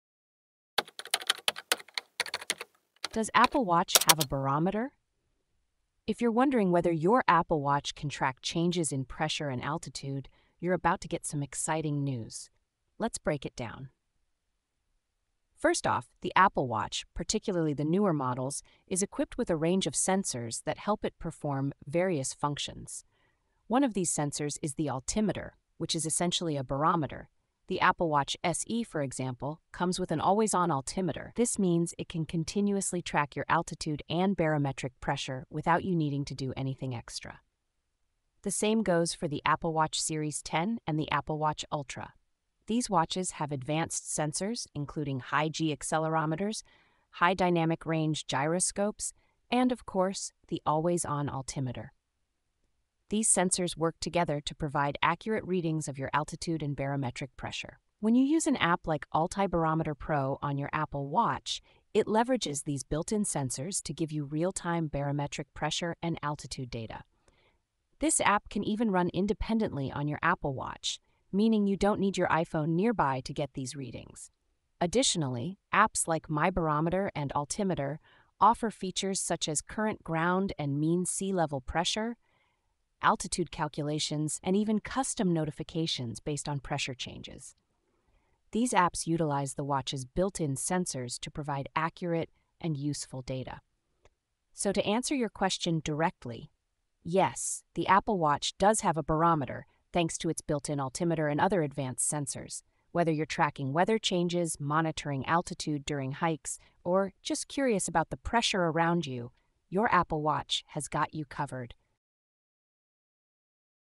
Does Apple Watch Have A Barometer? - Talking Tech Trends
Does Apple Watch Have A Barometer? Are you curious about the features of the Apple Watch, particularly its ability to track changes in pressure and altitude? In this informative video, we’ll discuss the various sensors integrated into the Apple Watch that make this possible. We’ll highlight the altimeter, which functions as a barometer, and explore how it operates seamlessly in the latest models, such as the Apple Watch SE, Series 10, and Apple Watch Ultra.

We’ll also introduce you to some of the most popular apps available for the Apple Watch that utilize these sensors, allowing you to monitor real-time barometric pressure and altitude. Whether you’re hiking, tracking weather changes, or simply interested in the atmospheric conditions around you, the Apple Watch is designed to provide you with accurate data at your fingertips.

Join us as we uncover the capabilities of the Apple Watch and how it can enhance your daily activities.